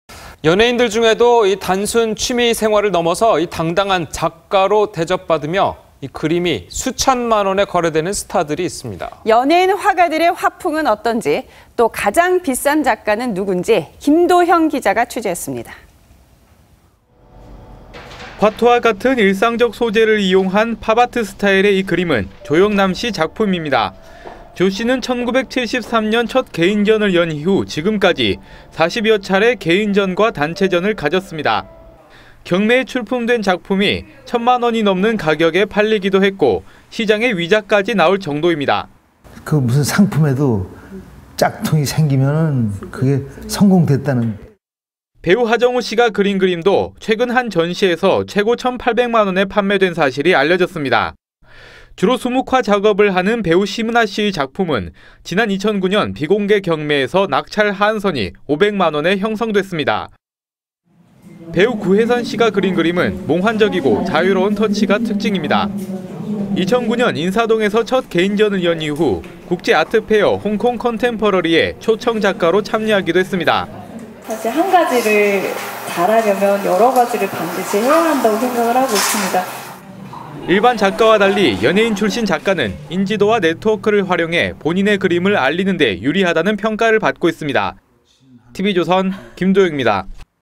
연예인 그림값 순위는?
연예인들 중에도 단순 취미 생활을 넘어 당당한 작가로 대접받으며 그림이 수천만원에 거래되는 스타들이 있습니다. 연예인 화가들의 화풍은 어떤지, 또 가장 비싼 작가는 누군지